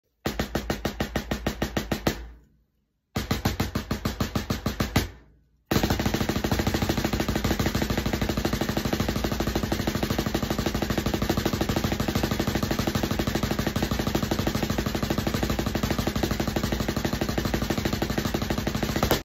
Finger Control Exercise!
Working the small gears! Trying to use the fingers to keep Propelling the stick. Playing off of the rebound and keeping it going. It’s a great way to build up the smaller fast twitch muscles. Start slow and gradually build up the speed. It’s all about accepting the rebound and only throwing the stick down using the fingers.